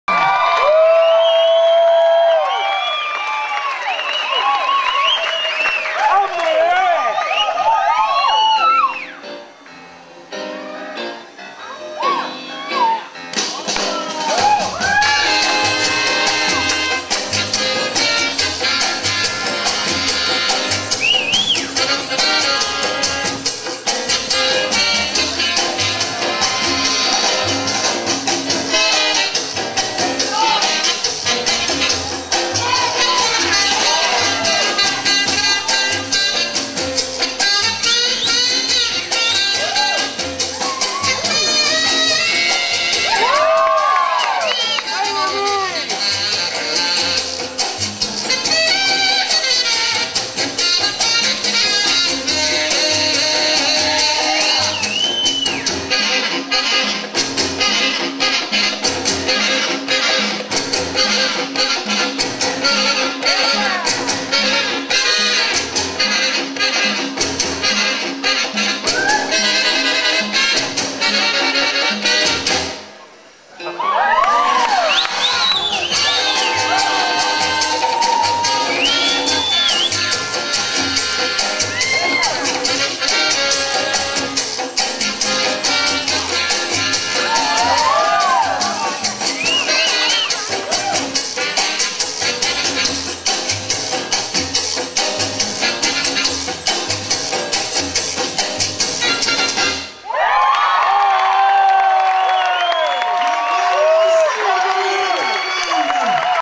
Yvonne G. Laycy. Cha Cha Solo - Dou Dou Le Dance Camp 2010
Yvonne G. Laycy. (formerly Yvonne Goldsmith) Cha Cha Solo - Dou Dou Le Dance Camp 2010. Shines, footwork & ladies styling